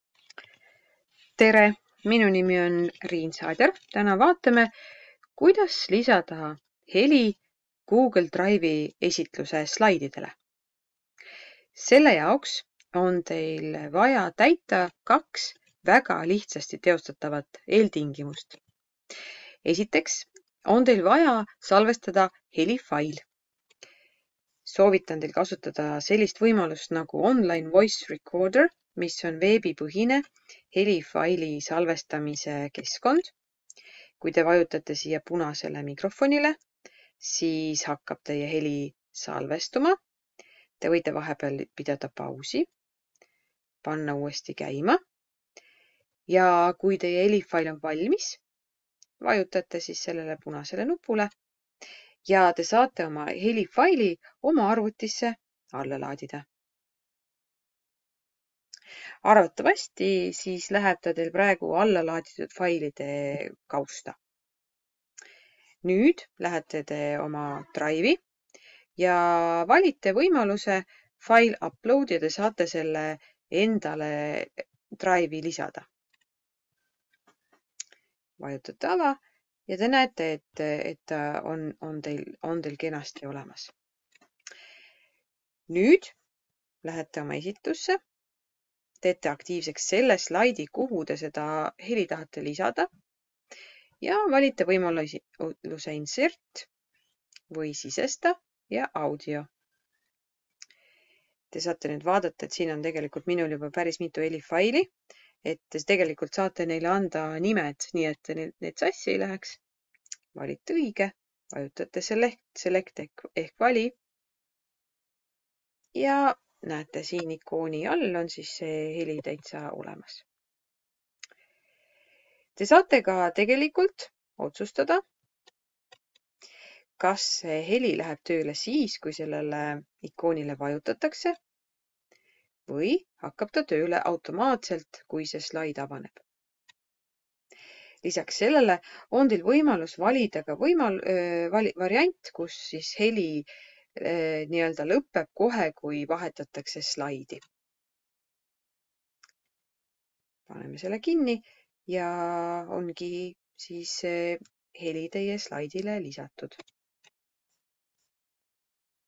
Heli Google slaididele
Puhja Gümnaasiumi haridustehnoloog ja inglise keele õpetaja Riin Saadjärv näitab, kuidas lisada Google Drive esitluse salididele heli. Helindamine on hea võimalus pakkuda slaidil olevale lisaselgitusi. Nii saab õpilast kaasata ja olla tema jaoks rohkem kohal.